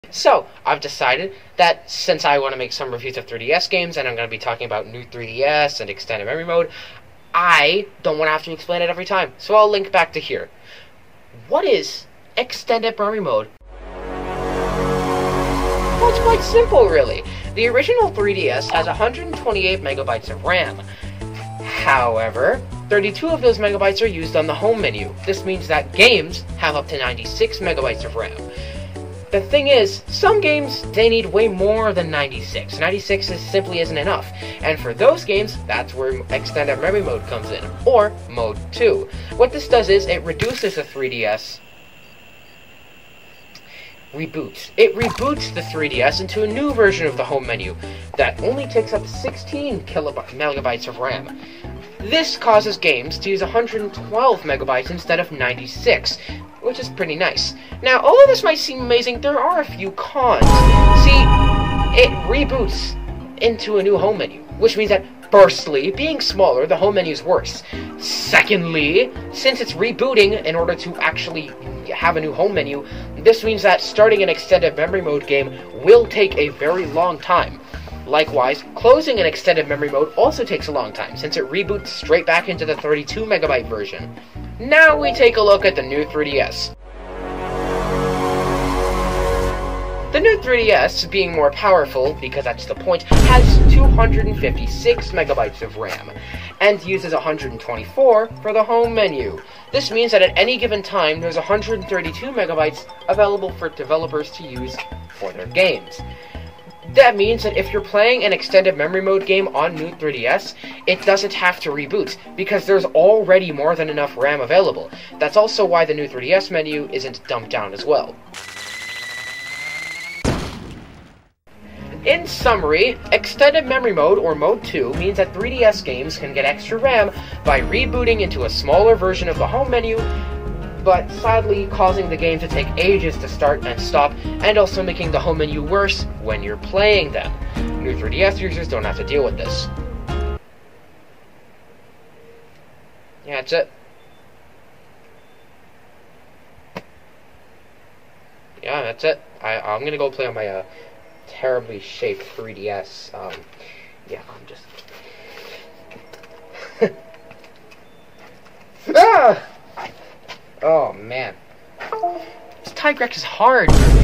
What is 3DS Extended Memory Mode?
I want to make 3DS game reviews and some of them use extended memory mode. Instead of explaining every time, I'll just link to here. Easy!